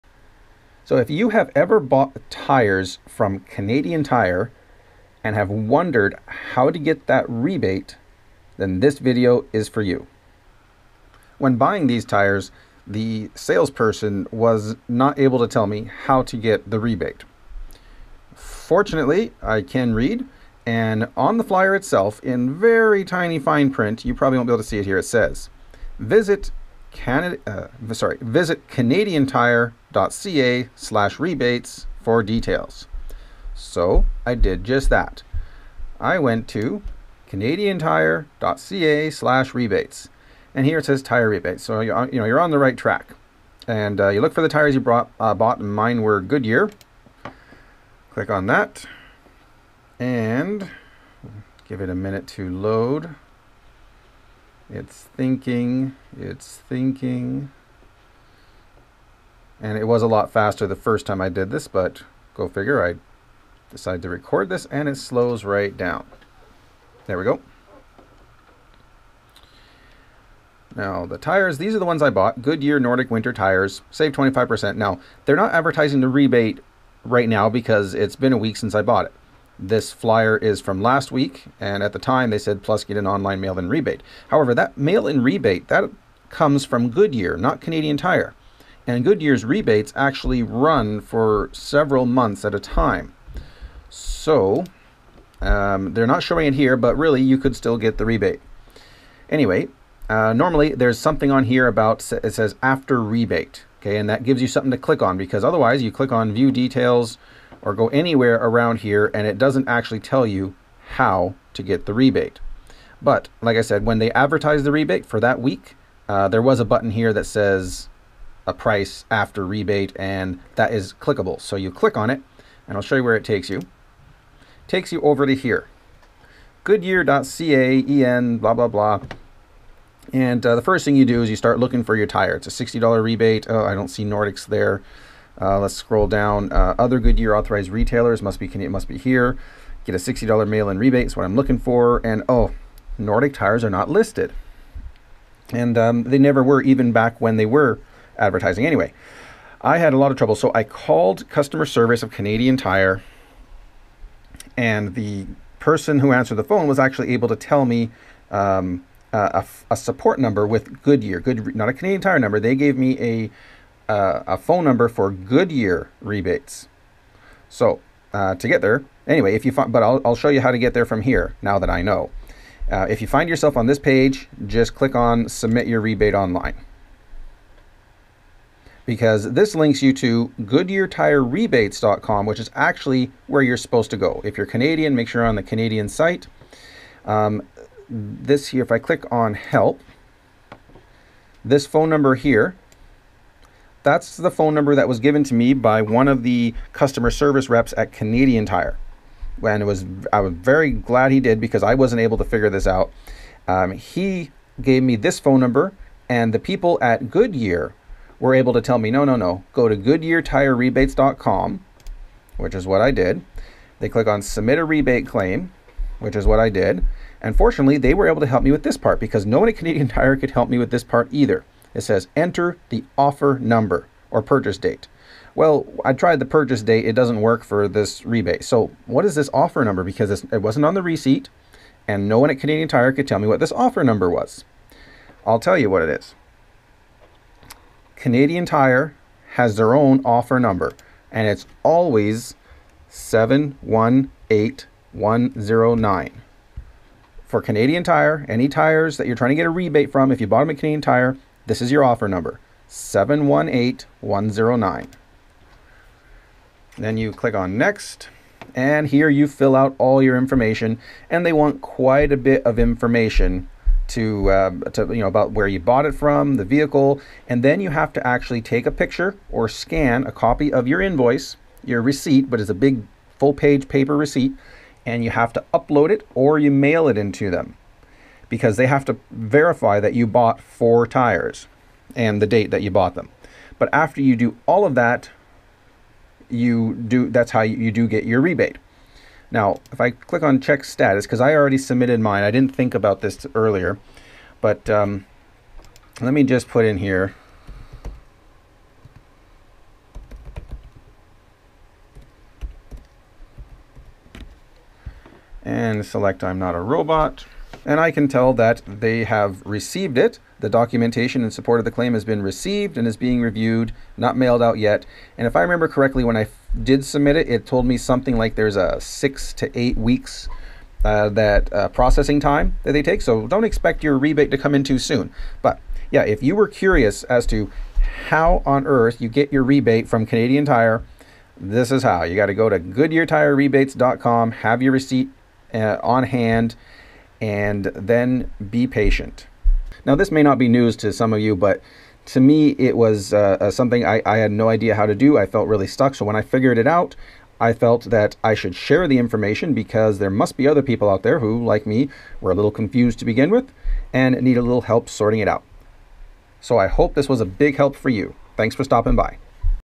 How to get your Goodyear tire rebates from Canadian Tire
In my first experience of applying for an online/mail in rebate for tires, I discovered it isn't as straightforward as one might expect.  I bought Goodyear Nordic tires from Canadian Tire and had to call multiple places in order to figure out how to get the rebate.

Another thing I discovered is the offer number for Canadian Tire.  It isn't on the receipt and nobody at the store seems to know it.  When you go to apply for your rebate you will need Canadian Tire's offer number.

UPDATE:
On November 23, 2018 I got the rebate in the form of a prepaid mastercard for $60.